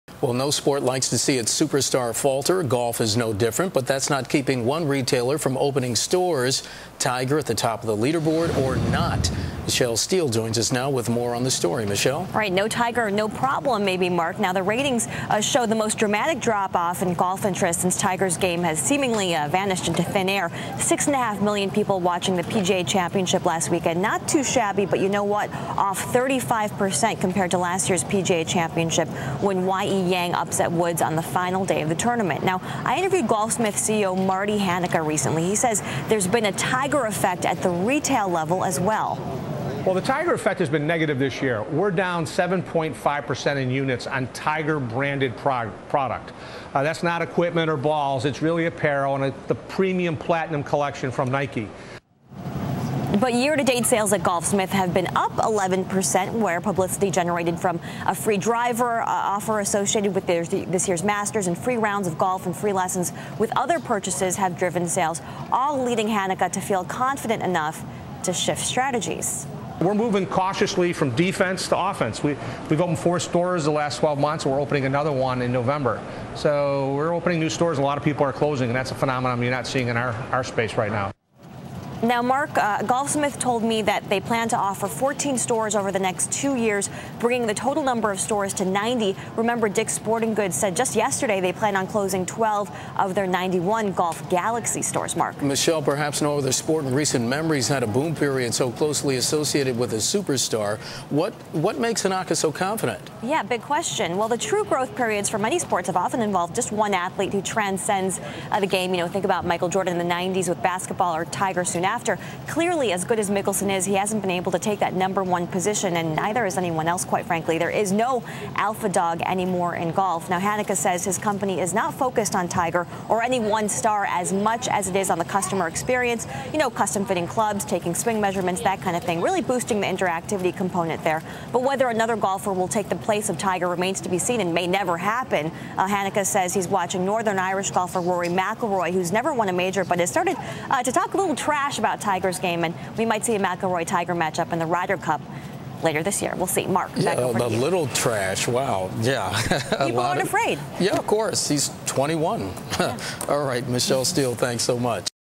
Golfsmith's Hanaka
Aug. 20 (Bloomberg) -- Martin Hanaka, chief executive officer at Golfsmith International Holdings Inc., talks with Bloomberg's Michele Steele about the sales of Tiger Woods-Branded products and the company's business plan.
     Golfsmith retails golf equipment, clothing and accessories through a chain of retail stores, catalogs and the Internet.  (Source: Bloomberg)